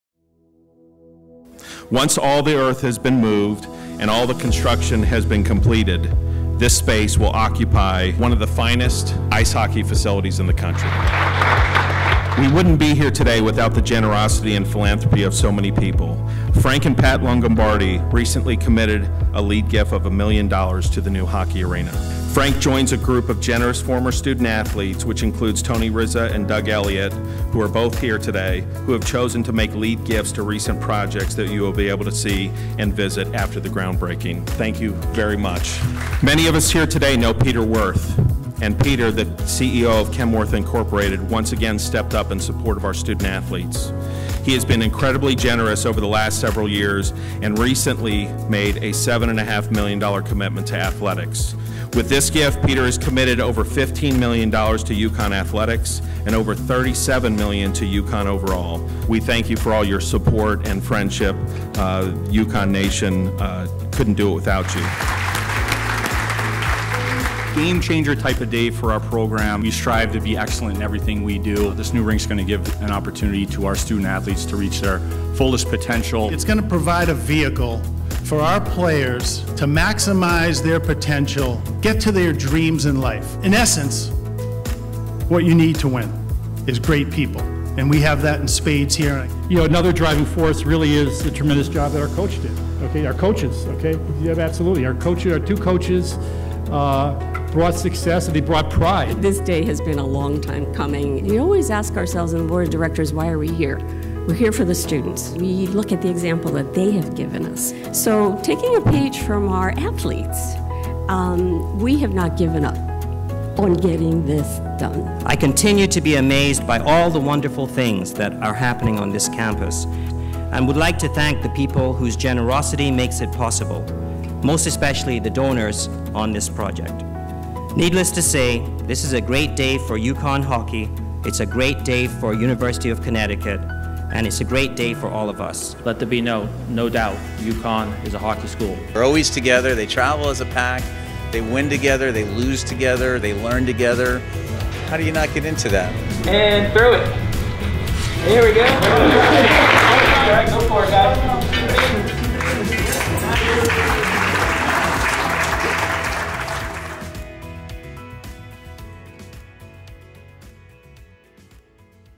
UConn Celebrates Groundbreaking for New Ice Hockey Arena
Saturday, May 22, 2021 will mark the beginning of a new era for the UConn men's and women's hockey programs with the official groundbreaking for the brand-new UConn Ice Hockey Arena on Jim Calhoun Way.

Joining current and former hockey student-athletes were Director of Athletics David Benedict and head coaches Mike Cavanaugh and Chris MacKenzie as well as new UConn Interim President Dr. Andre Agwunobi, UConn Board of Trustee Chairman Dan Toscano '87 and fellow Board of Trustee members Thomas Ritter, Esq., '77 and Marilda Gandara, Esq. '78 as well as Hockey East Association Commissioner Steve Metcalf.